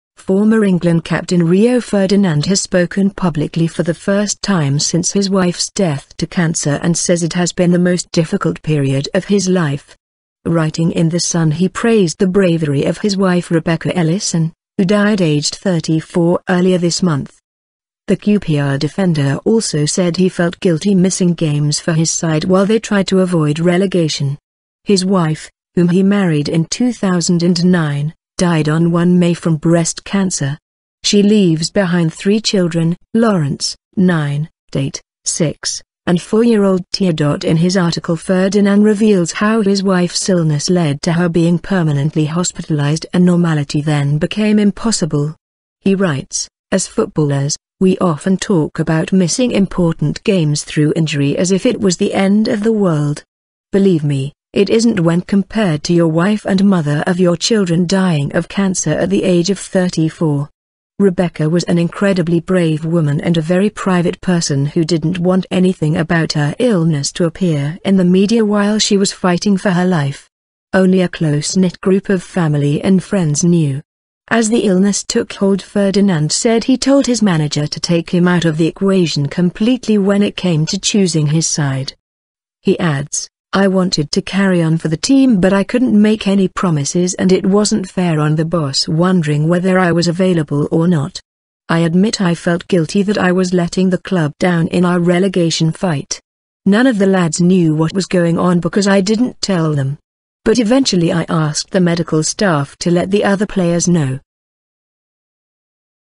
Rio Ferdinand speaks after wife's death from cancer
Former England captain Rio Ferdinand has spoken publicly for the first time since his wife's death to cancer and says it has been "the most difficult period of his life".
Writing in the Sun he praised the bravery of his wife Rebecca Ellison, who died aged 34 earlier this month.
The QPR defender also said he "felt guilty" missing games for his side while they tried to avoid relegation.
His wife, whom he married in 2009, died on 1 May from breast cancer.